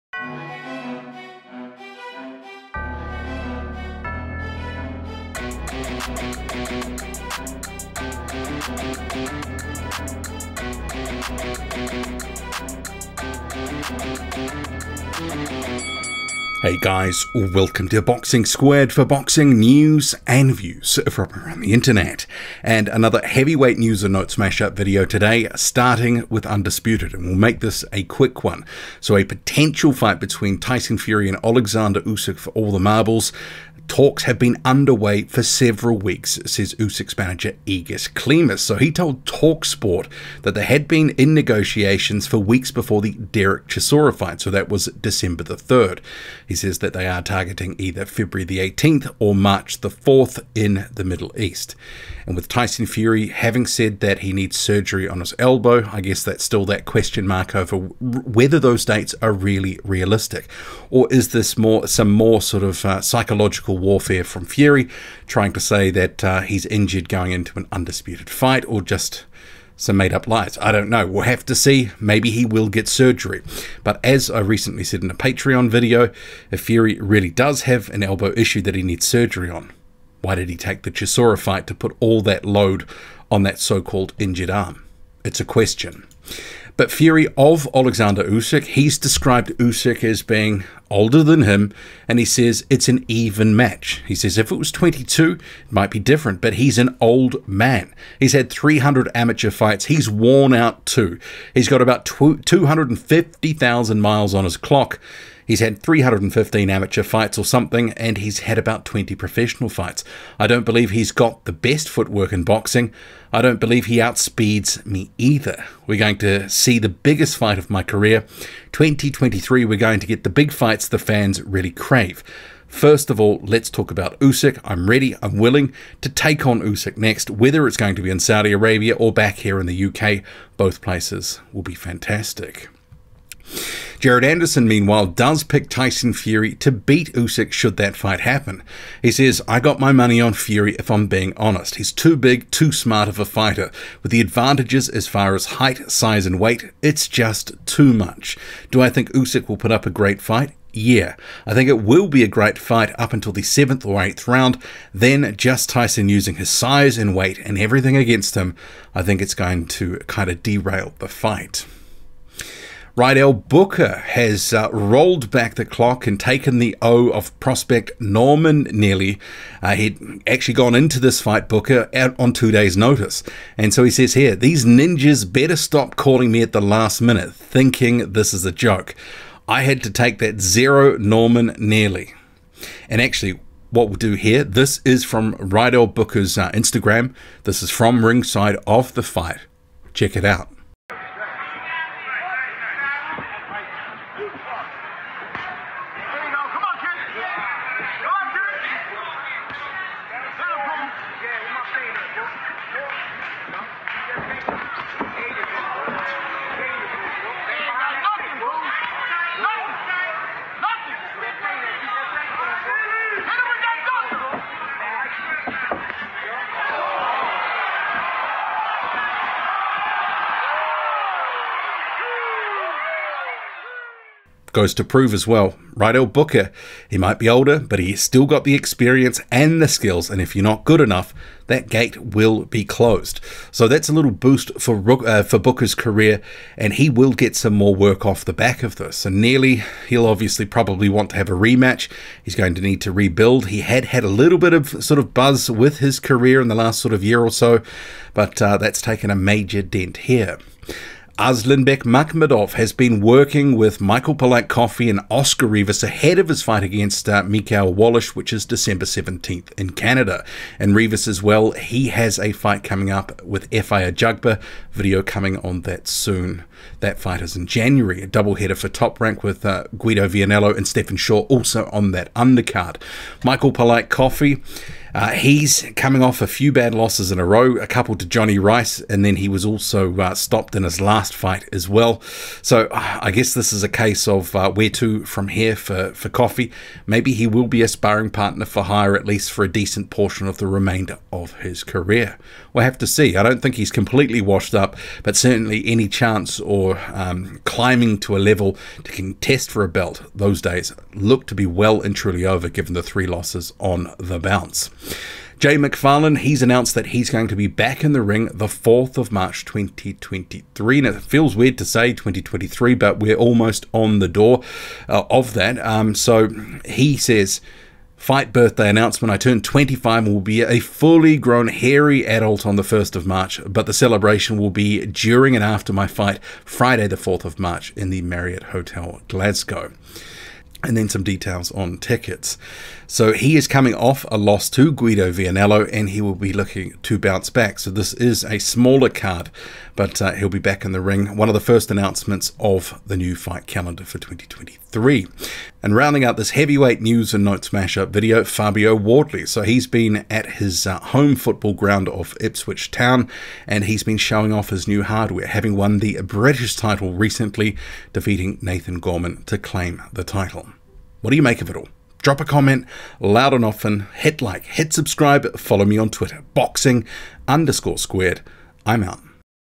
NEWS MASH UP: OLEKSANDR UYSK, TYSON FURY, RYDELL BOOKER & ARSLANBEK MAKHMUDOV
Join me, Boxing Squared, for a wrap of news & views from the heavyweight division. Featuring: Oleksandr Usyk, Tyson Fury, Jared Anderson, Rydell Booker, Norman Neely, Arslanbek Makhmudov, Michael Coffie, Oscar Rivas, Jay McFarlane, Fabio Wardley

Boxing Squared

World Heavyweight Boxing